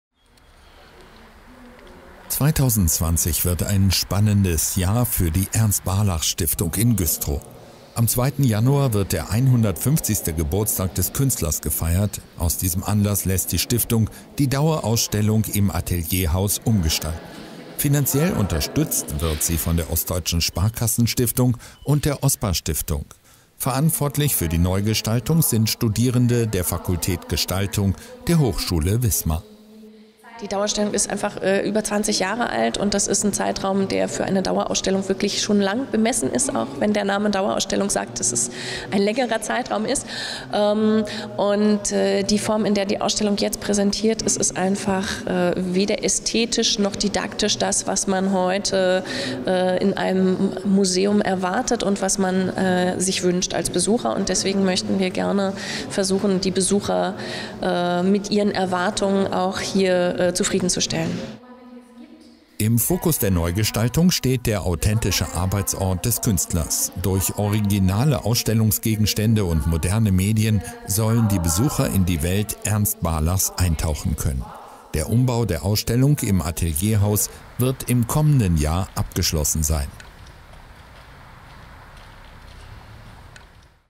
150 Jahre Ernst Barlach: Dauerausstellung im Atelierhaus in Güstrow wird erneuert
Mit dem 150. Geburtstag Ernst Barlachs am 2. Januar startet die Ernst Barlach Stiftung in ein spannendes Jahr. Denn 2020 soll die Dauerausstellung im Atelierhaus des deutschen Bildhauers erneuert werden. Die Ausstellung ist bereits über 20 Jahre alt. Für die Neugestaltung sind Studierende der Fakultät Gestaltung der Hochschule Wismar zuständig.